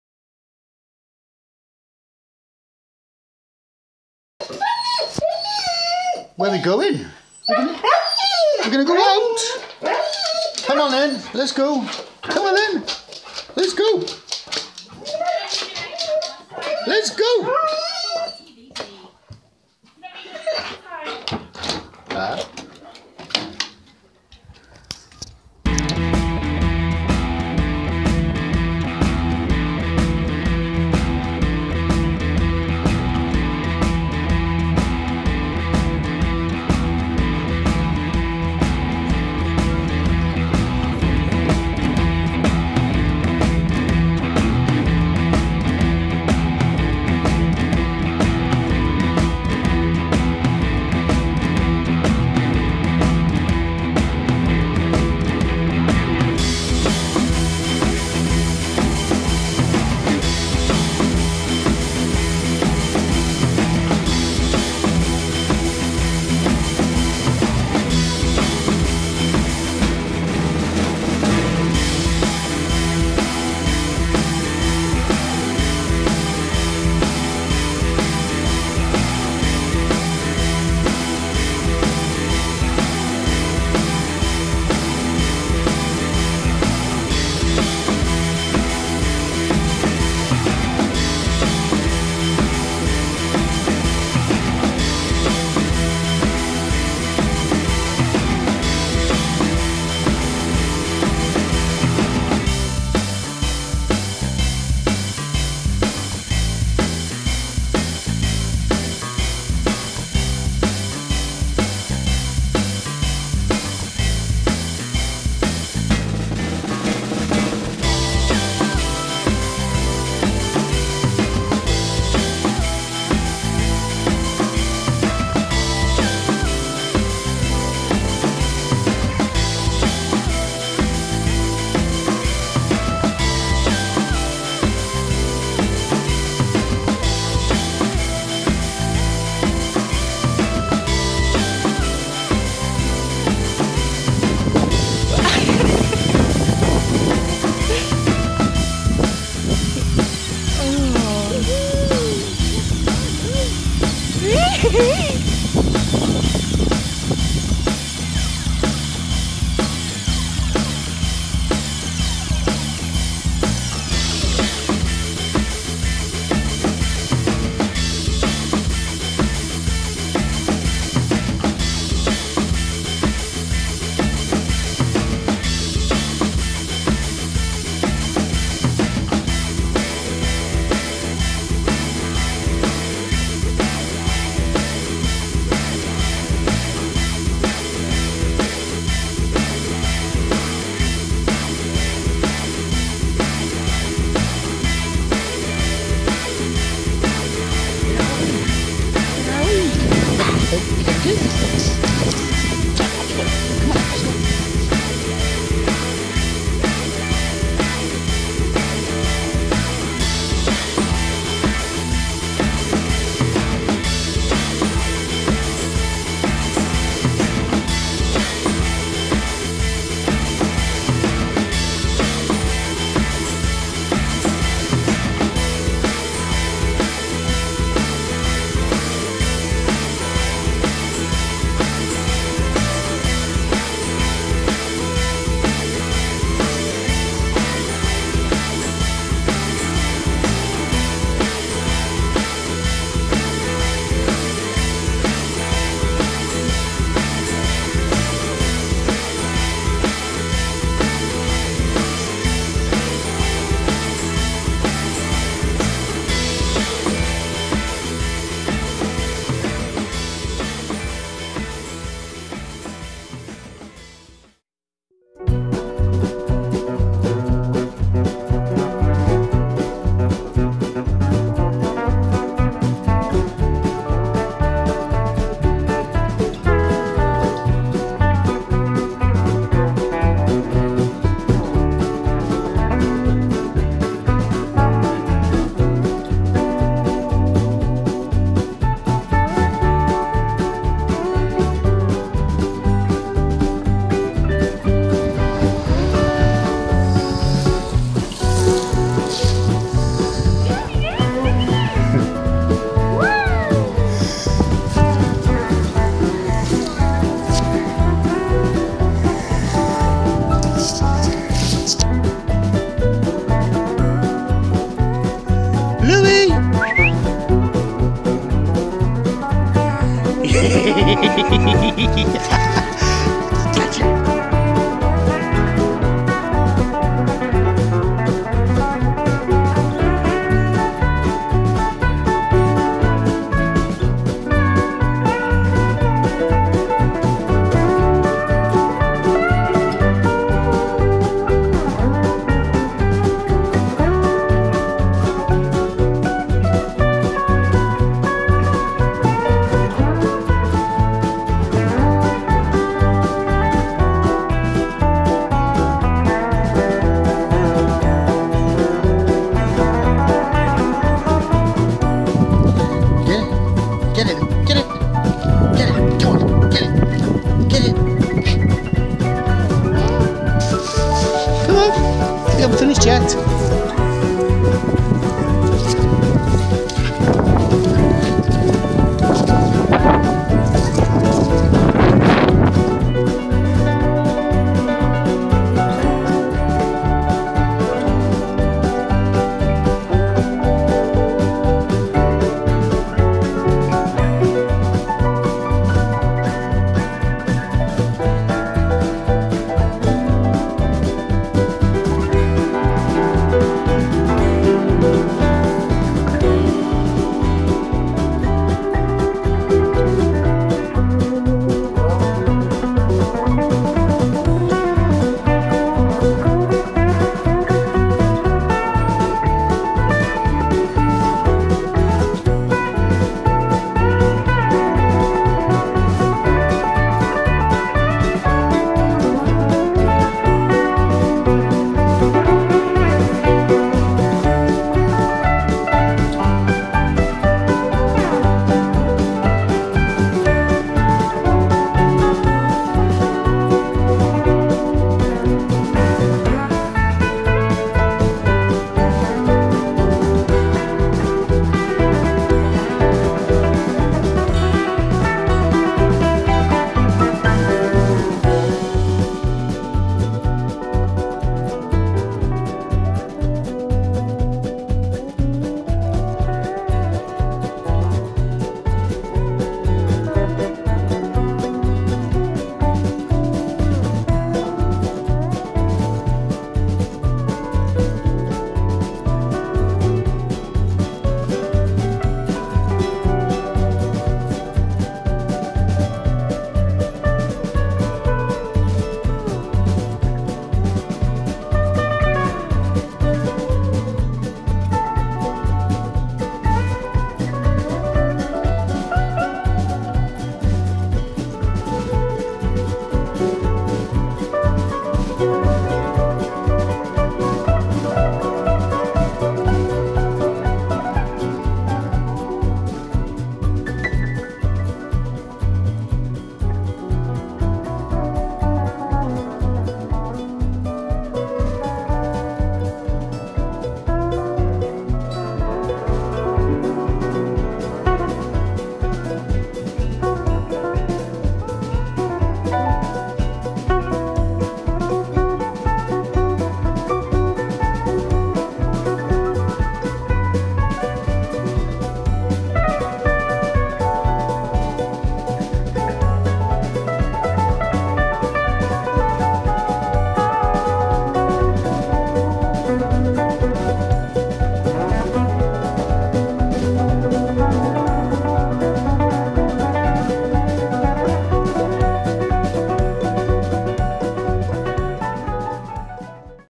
llanbradach fawr fields
a great business that delivers a secure and fun environment for reactive and nervous dogs to be allowed "off lead"